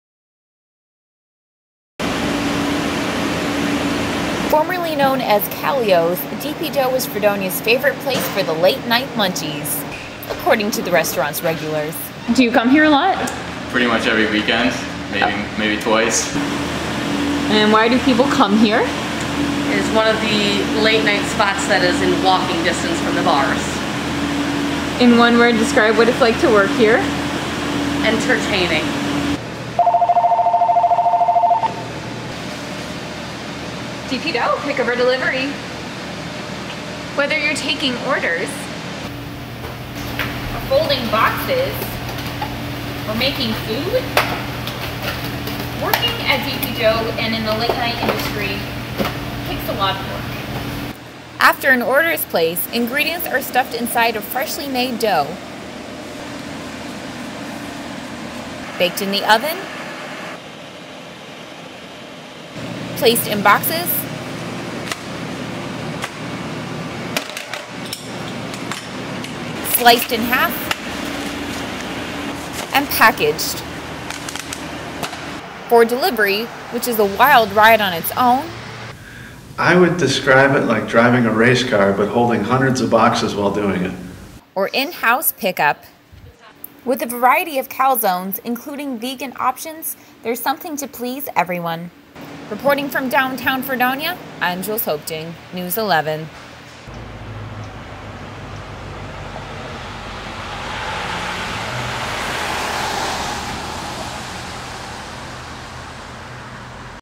D.P. Dough: Working in Late Night Food Delivery (News Package)
Final project for a news package class.
Reporter, script writer, videographer, video editor: Jules Hoepting.
Chyrons included in script.